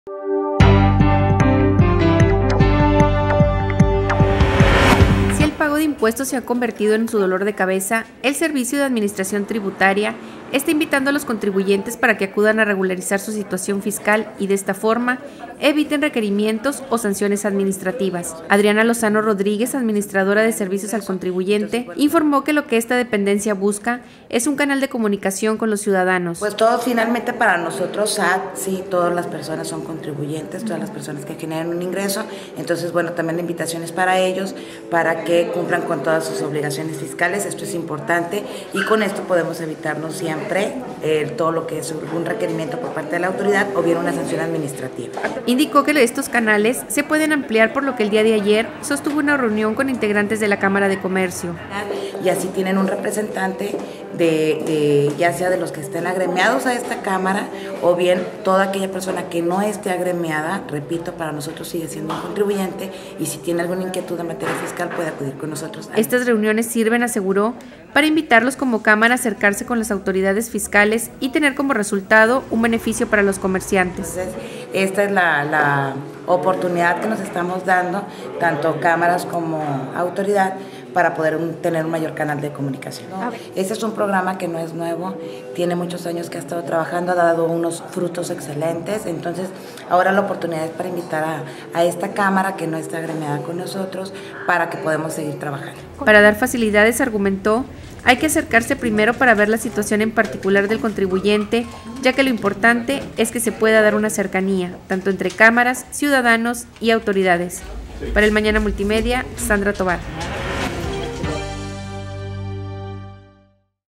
Invita SAT a contribuyentes evitar 'dolores de cabeza'
Si el pago de impuestos se ha convertido en su "dolor de cabeza", el Servicio de Administración Tributaria (SAT) está invitando a los contribuyentes para que acudan a regularizar su situación fiscal y de esta forma, evitar requerimientos o sanciones administrativas.
Adriana Lozano Rodríguez, Administradora de Servicios al Contribuyente Reynosa del SAT informó que lo que esta dependencia busca es un canal de comunicación con los ciudadanos.
"Para el SAT todas las personas son contribuyentes, todos los que generen un ingreso, por lo que la invitación es para que cumplan con todas sus obligaciones fiscales y con esto podemos evitarnos un requerimiento por parte de la autoridad o una sanción administrativa", dijo la funcionaria.
Indicó que los canales de comunicación se pueden ampliar, por lo que el día de hoy, sostuvo una reunión con integrantes de la Cámara de Comercio.
"Si tienen un representante, ya sea de los agremiados a la cámara o de todo aquel que no esté agremiado, para nosotros siguen siendo contribuyentes y si tienen alguna inquietud en materia fiscal pueden acudir a nosotros".
Estas reuniones sirven aseguró, para invitarlos como cámara a acercarse con las autoridades fiscales y tener como resultado un beneficio para los comerciantes.
Para dar facilidades argumentó, hay que acercarse primero para ver la situación en particular del contribuyente ya que lo importante es que se pueda dar una cercanía, tanto entre cámaras y ciudadanos con la autoridad.
"Es una gran gama de información fiscal aquí la indicación es que visiten nuestra página o bien si quiere obtener alguna asesoría u orientación fiscal, hay que resaltar la importancia de que todos los servicios son gratis, lo púnico es que tiene que accesar a la pagina e invocar al ícono visitas, generar la cita y confirmarla y acudir a servir estos servicios", concluyó.